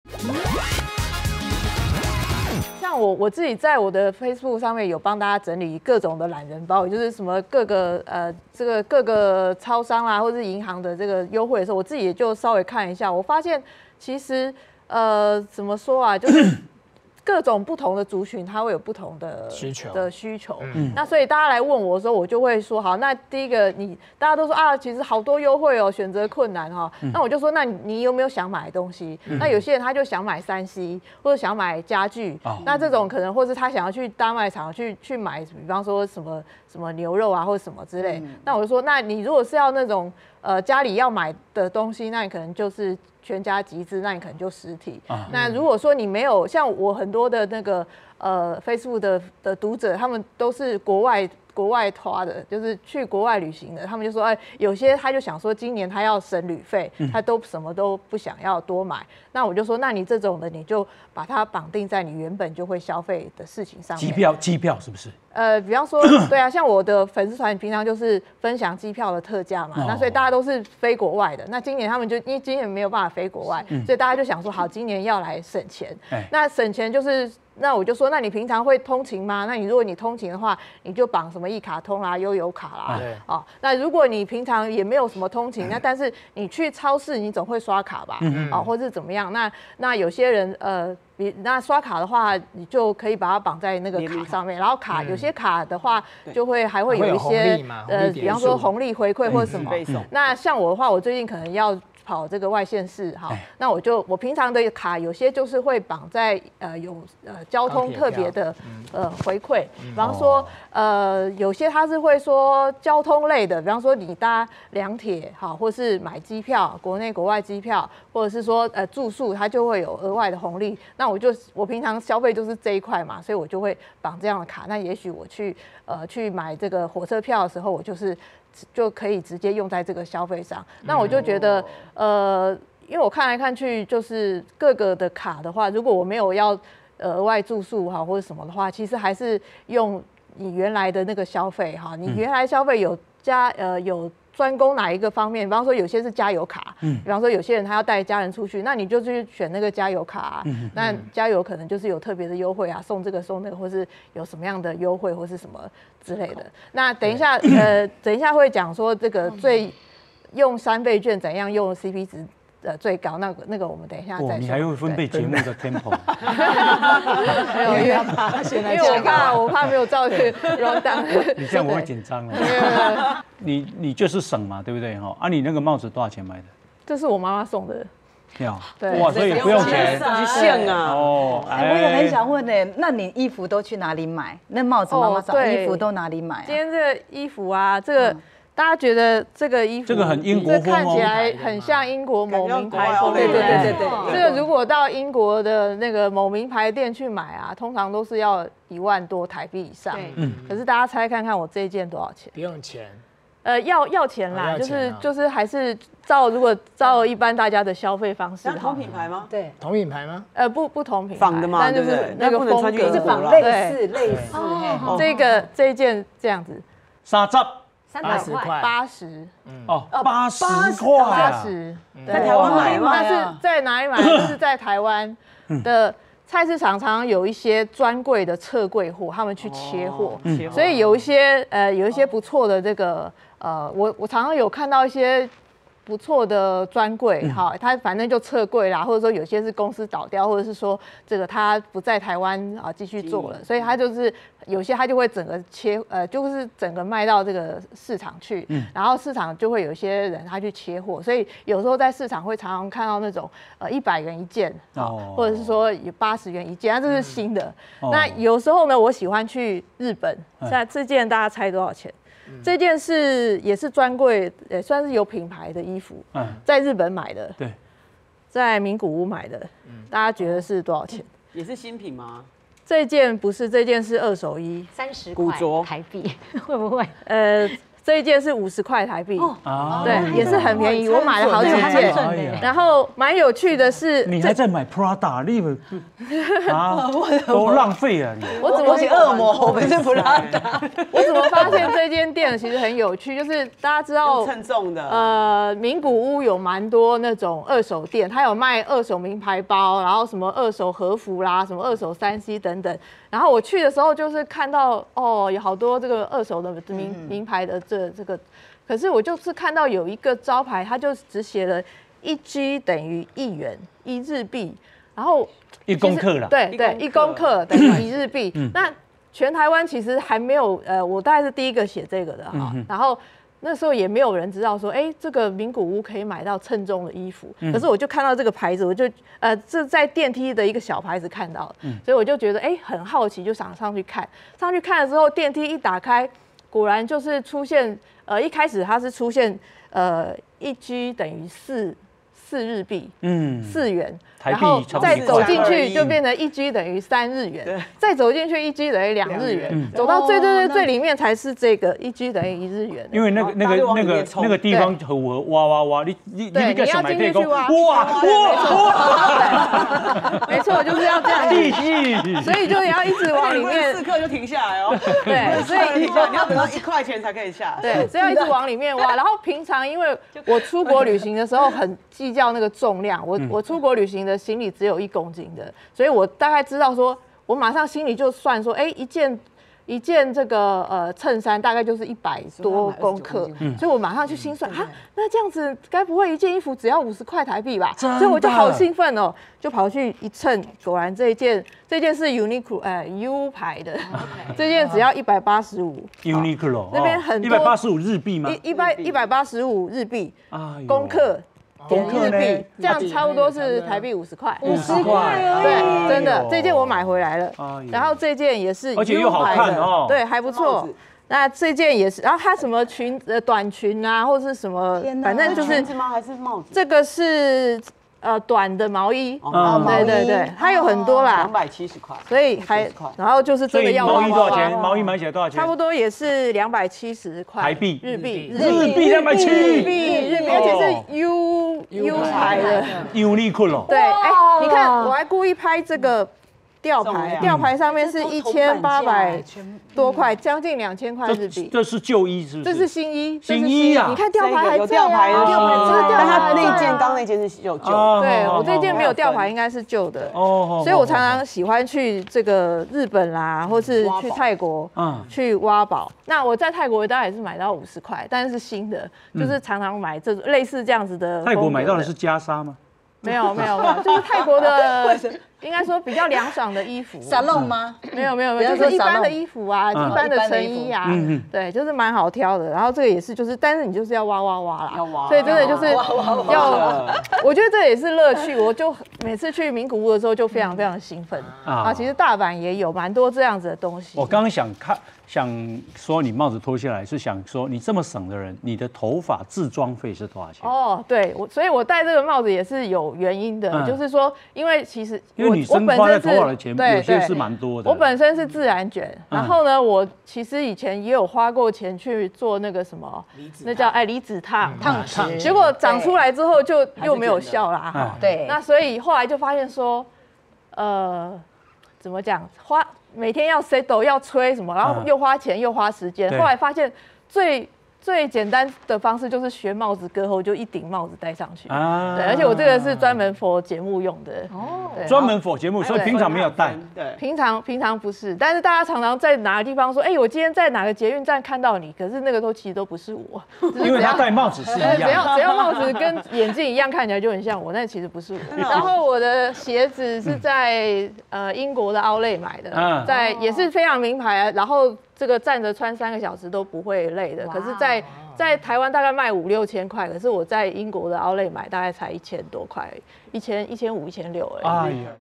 【精華版】消費達人帶你撿便宜，銅板價就能買進品牌服飾！
喜歡旅遊的943會在各地挖寶，從台灣菜市場買到國外秤重拍賣。